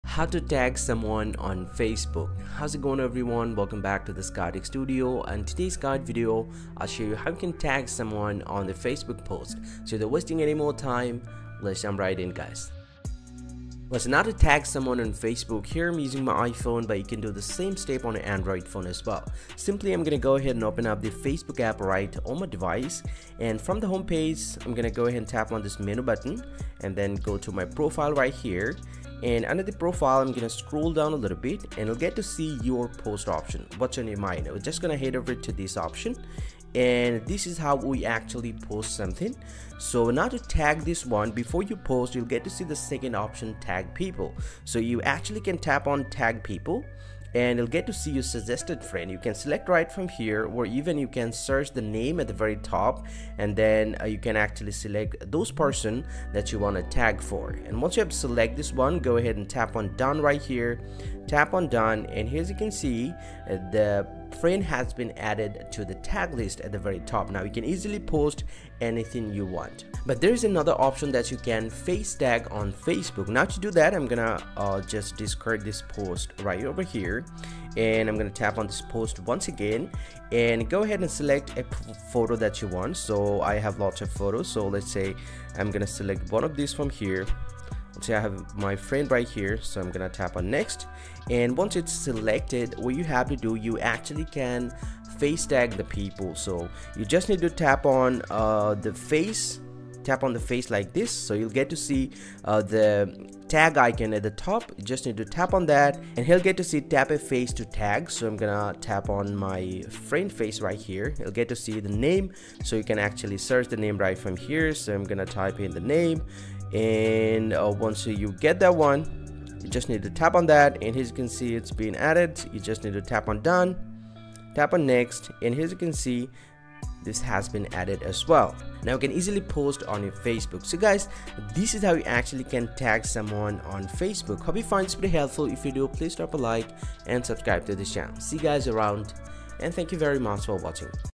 How To Tag Someone on a Facebook Post 2024 | How To Face Tag on Facebook
Facebook tag
How to face tag on Facebook
How to tag someone on a Facebook post

Learn how to tag someone on Facebook post. If you want to tag or Face tag your Facebook friends and ask How do I tag someone in a Facebook post? Here is the
step-by-step guide.

00:01- Video intro
00:13- Process 1: Tag Someone
01:10- Process 2: Face Tag Someone
02:15- Process Ending